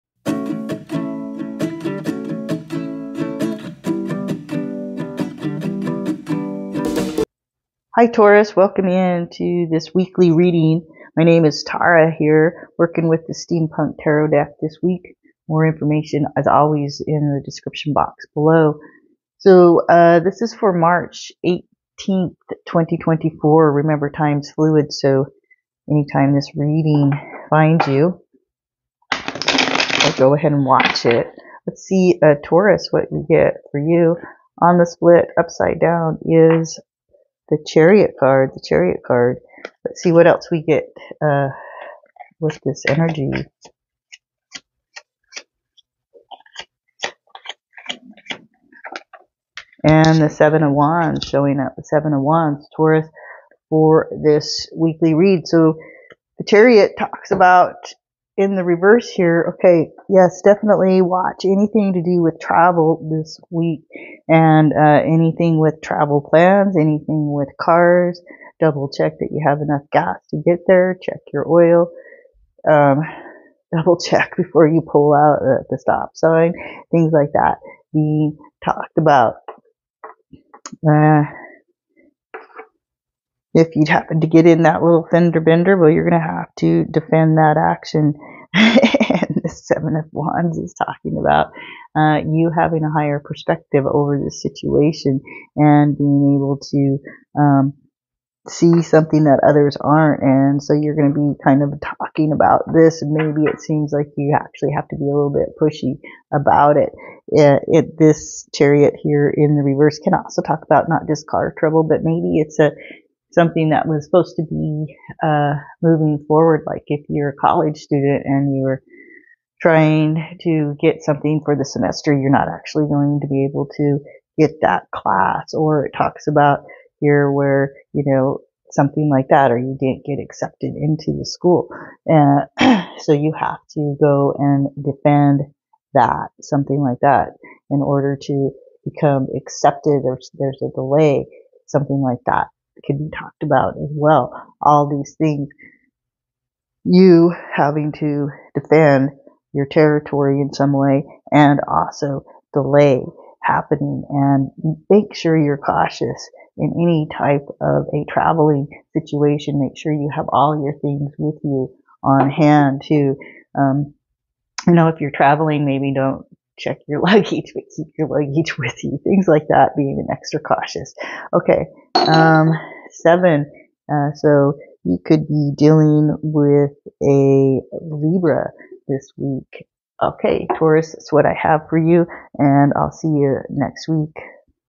Taurus Weekly Tarot March 18, 2024
Tara reads two cards for this weekly tarot reading, plus the role of the die to see what sign you are working with. Current energy for the week of March 18, 2024. Reading for the Zodiac Sign of Taurus. Time is flexible, so watch video when it fines you. Also, you don't have to be a Taurus to appreciate this video.

Reading from
Steampunk Tarot
Illustrations by
Aly Fell
Book by
Barbara Moore